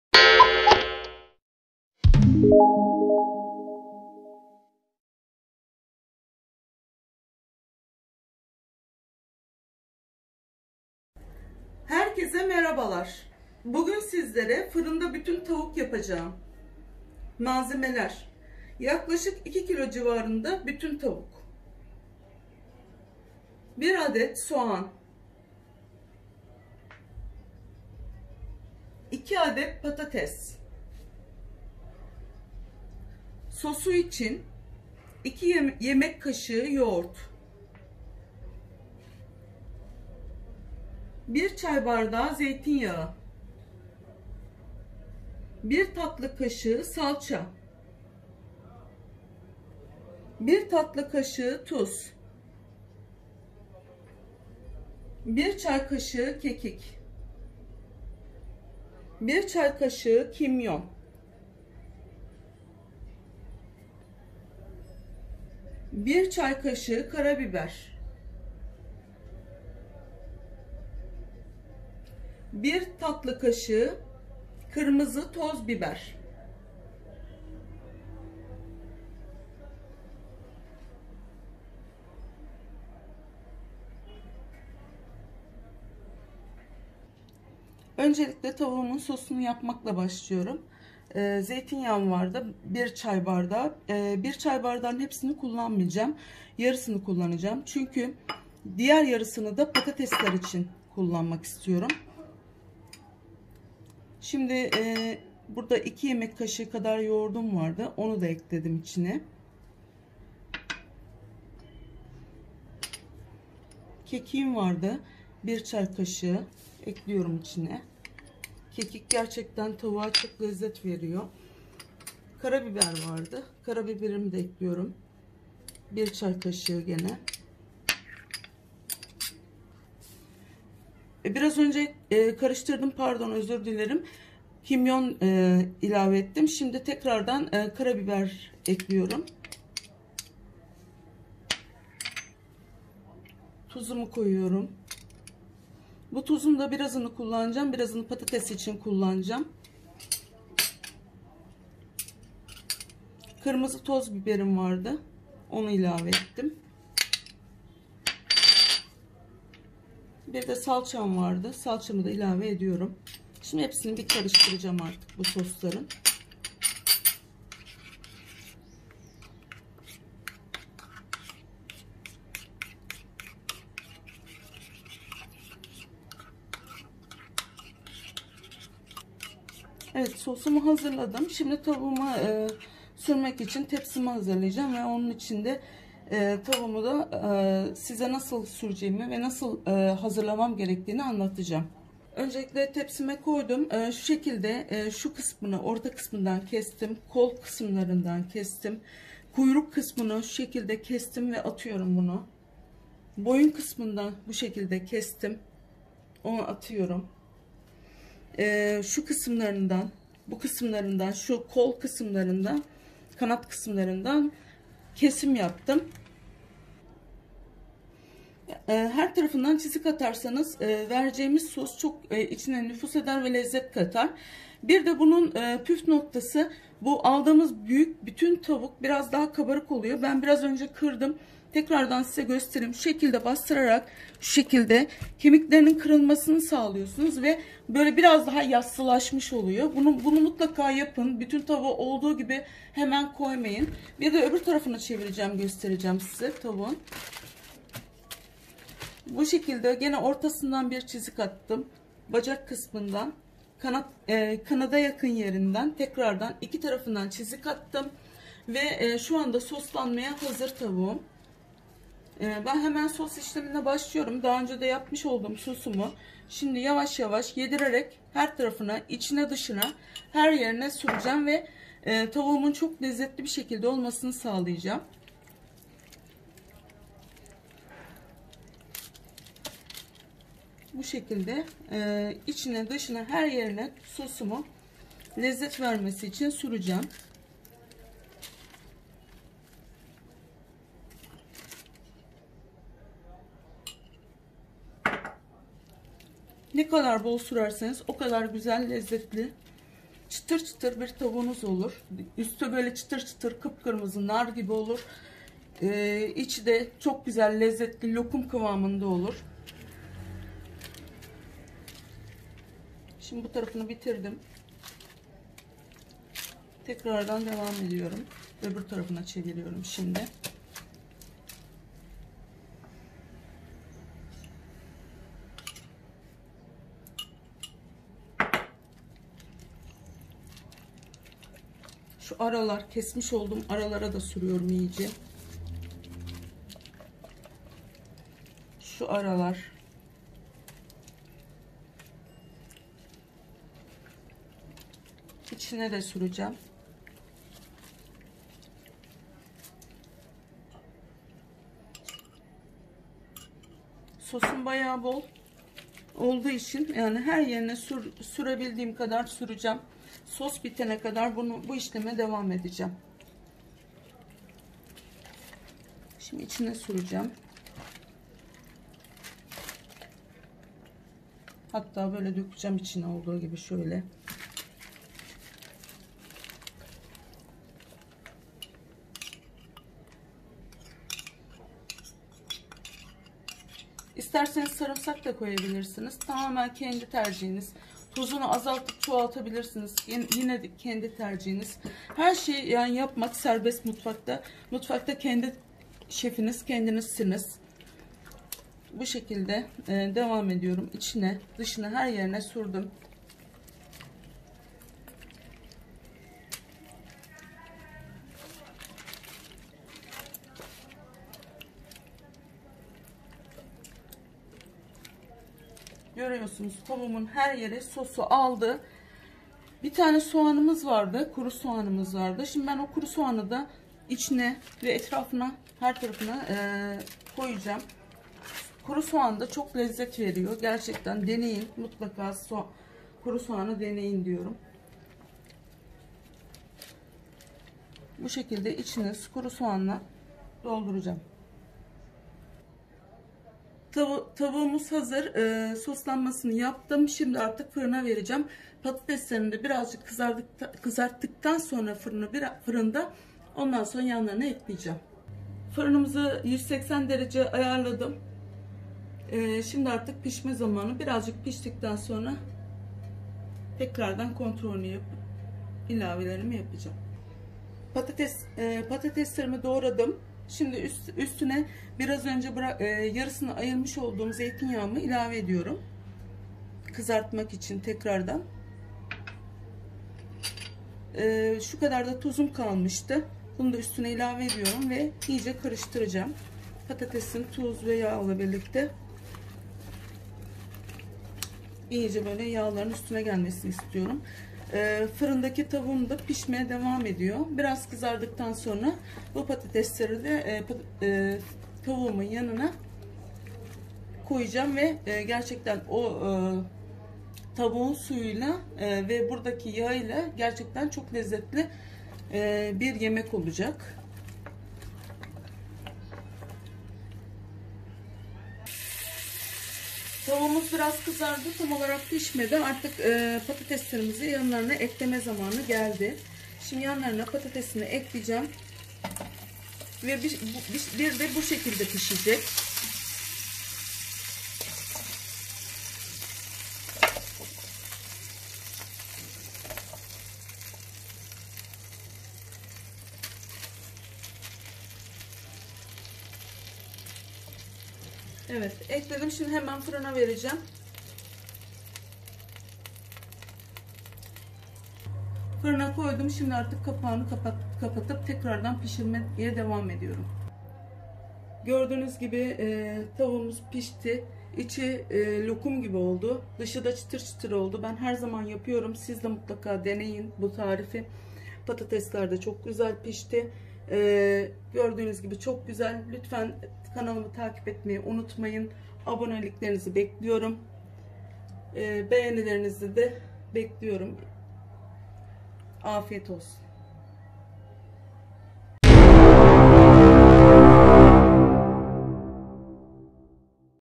Fırında Kızarmış Bütün Tavuk Tarifi / Fırında Bütün Tavuk Nasıl Pişirilir? /Fırında Bütün Tavuk
Malzemeler:
1 bütün tavuk
2 adet büyük patates
1 adet kuru soğan
Sosu için:
1 çay bardağı zeytinyağı
1 tatlı kaşığı salça
2 yemek kaşığı yoğurt
1 tatlı kaşığı tuz
1 çay kaşığı kekik
1 çay kaşığı karabiber
1 çay kaşığı kimyon
1 tatlı kaşığı toz kırmızı biber

Afiyet olsun...
Gerçek Lezzetler

Materials:

1 whole chicken
2 potatoes
1 onion
 For the sauce:
1 tea glass of olive oil
1 sweet spoon of tomato paste
2 tablespoons of yogurt
1 sweet spoon of  salt
1 teaspoon of thyme
1 teaspoon of blackpepper
1 teaspoon of cumin
1 sweet spoon of ground red pepper

Bon Appetit..